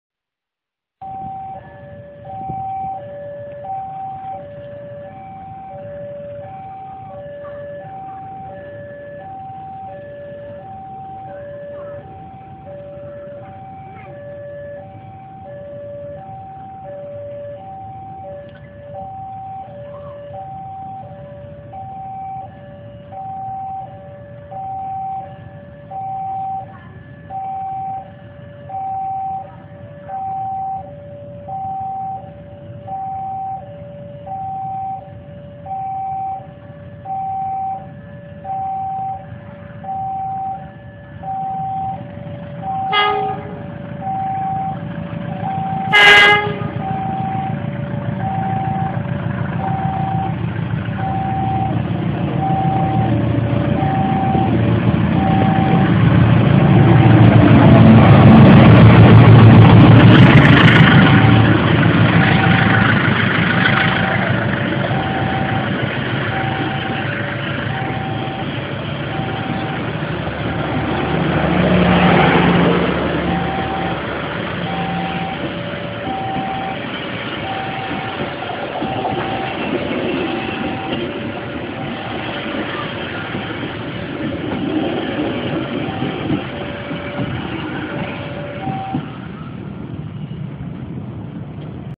Kereta Api Brantas (Kediri-Pasar Senen) berangkat dari Stasiun Solo Jebres
Kereta Api Brantas dari Kediri tujuan akhir Pasar Senen berangkat dari jalur 3 Stasiun Solo Jebres. Video diambil pada 22 September 2015, 17.01 WIB